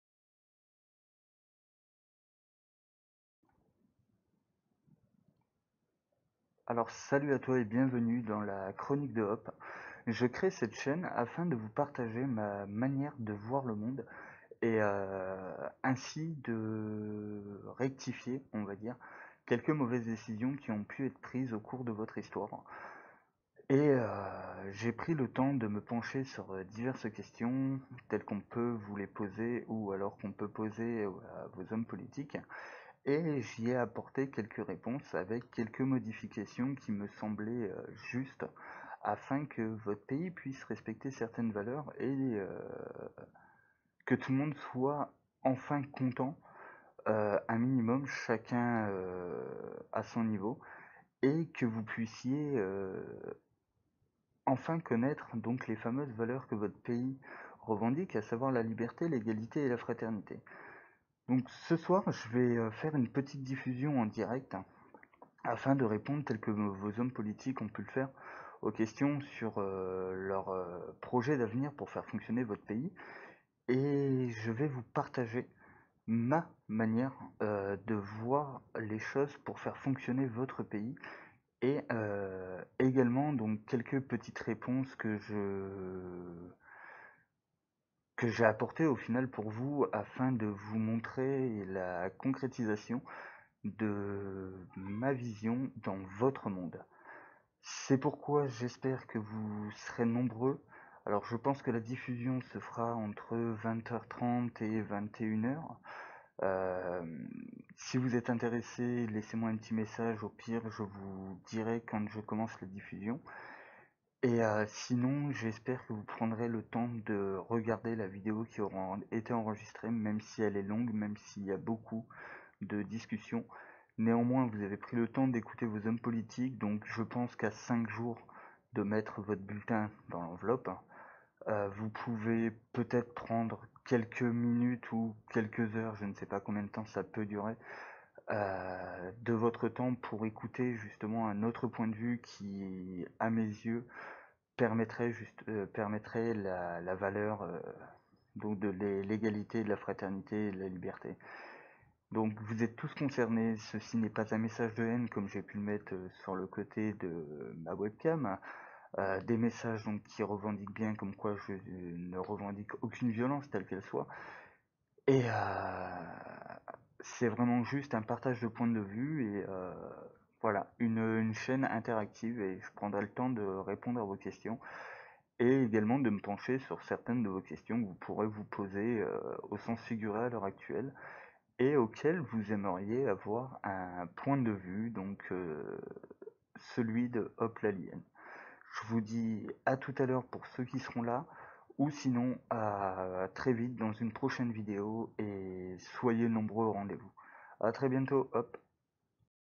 La chronique de Hope.( Présentation )
Bonjour,
Bienvenu dans la chronique de Hope.
Je te propose de répondre à certaines questions mais aussi te donner mon avis sur tout et rien.

Retrouve moi sur snap: Hope-world